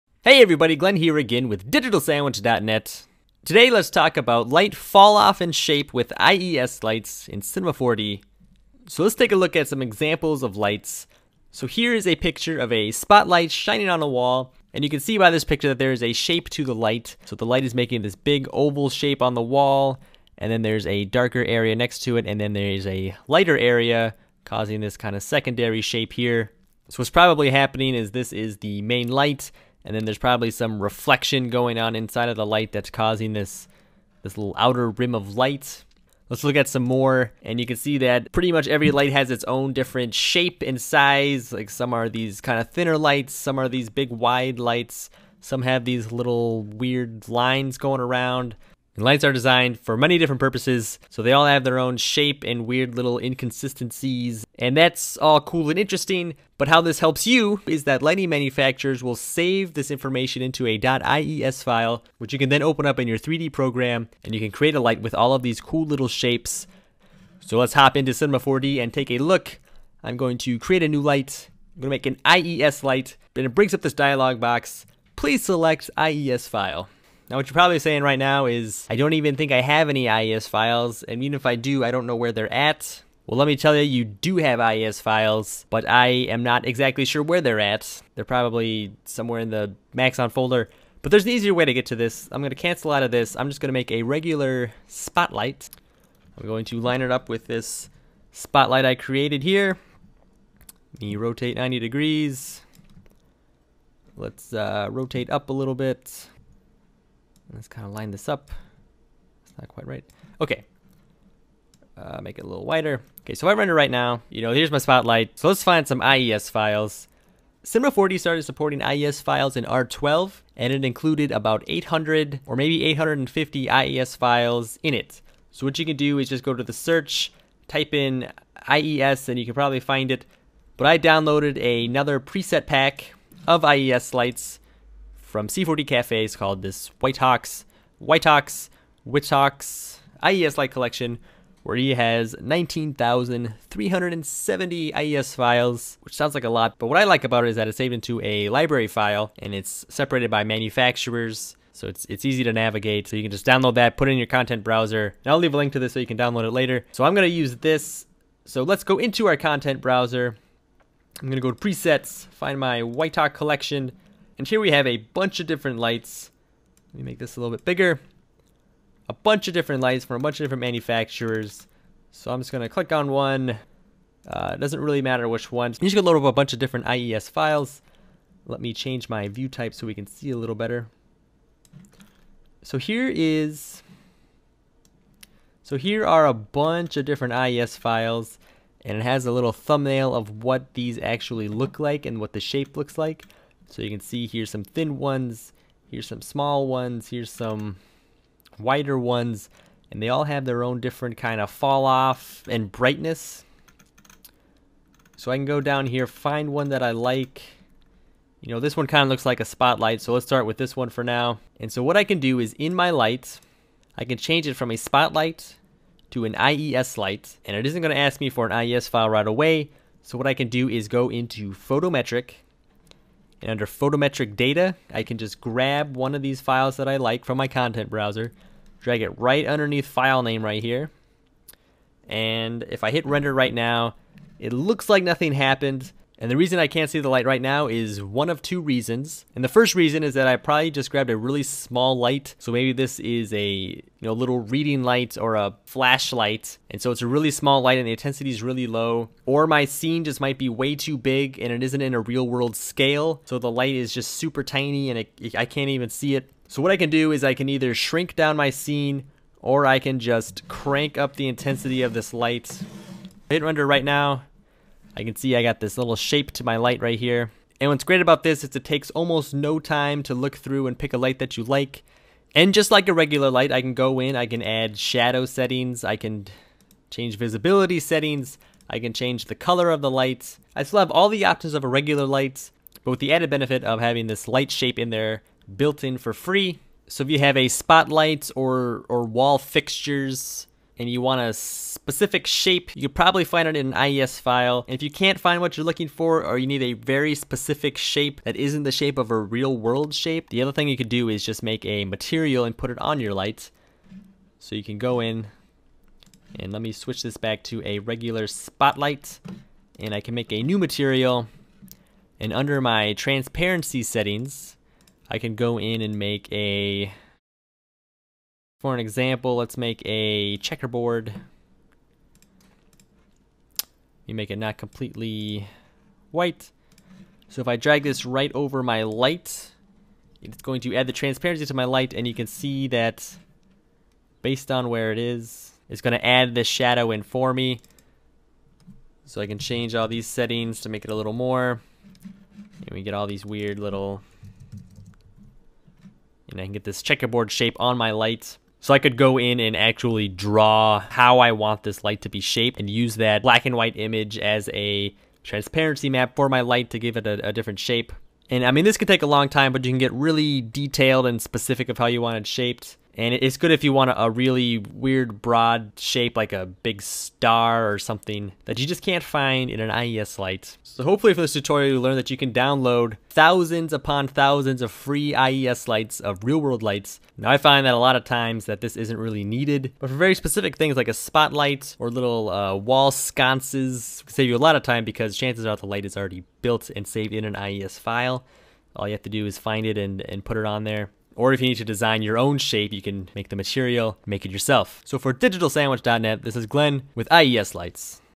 Cinema 4D Tutorial - Realistic Light Falloff with IES Lights in C4D
This tutorial covers how to get realistic light falloff and shape with IES lights in Cinema 4D. The guys from digitalsandwich.net will show you how to set up your lights, how to get more .ies files, and finally how to create your own custom light shapes with a material.

0:00 - What are IES lights?
1:08 - Do I have any .ies files and where to get thousands more.
2:38 - How to find and setup your .ies files.
4:52 - How to create a custom light falloff and shape.

If you enjoyed this tutorial, make sure you check out digitalsandwich website and social media here